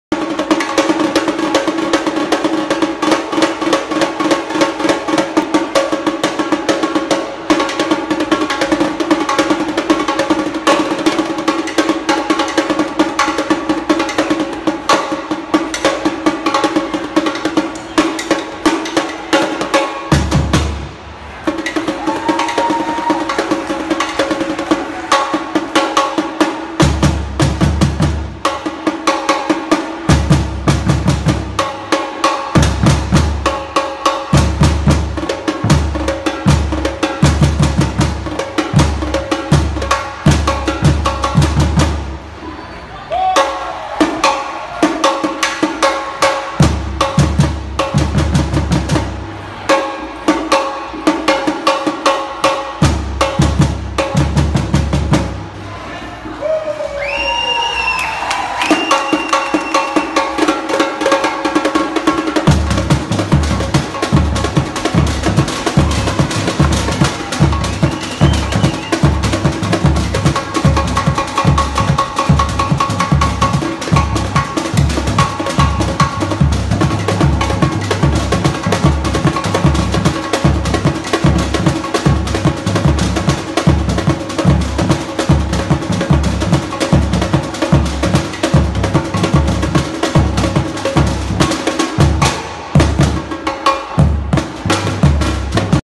Dr. Samba's Repinique Solo at Latin Fest in West Palm Beach, FL
Director of Samba New York!, Philip Galinsky, Ph.D. (a.k.a. Dr. Samba) takes a repinique solo during Samba New York's performance at Latin Fest at the South Florida Fairgrounds in West Palm Beach, FL on January 21, 2018.

Special thanks to Laura Macaddino (1st/2nd Surdo); Louis Winsberg (3rd Surdo); Everton Isidoro (Caixa); and Valerie Galinsky (Managing Director and videographer).